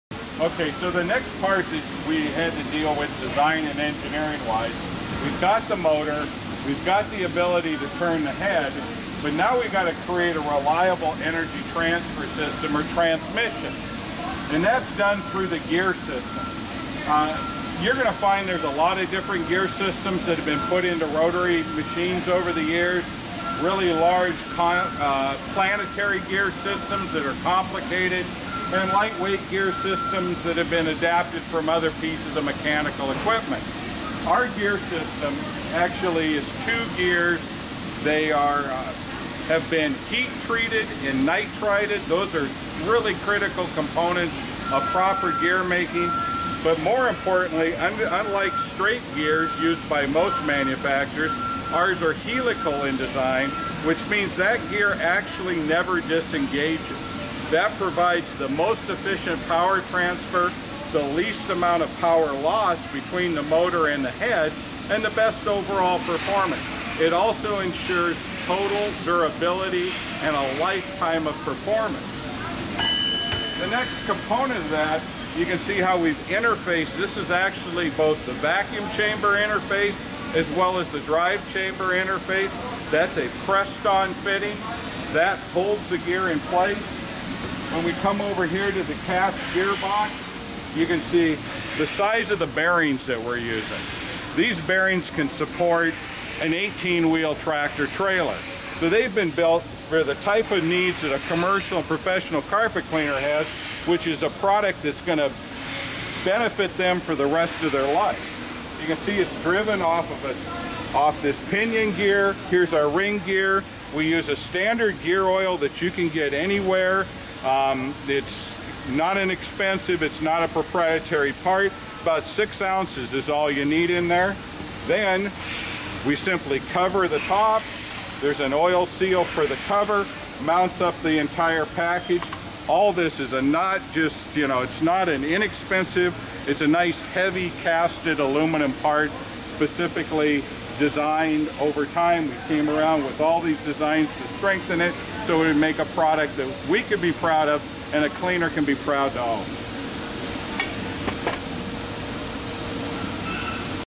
HOSS 700 - Gear & Bearing
Legend Brands CEO Bill Bruders overviews the HOSS 700 Rotary Cleaning Tool from Sapphire Scientific. Gear & Bearings.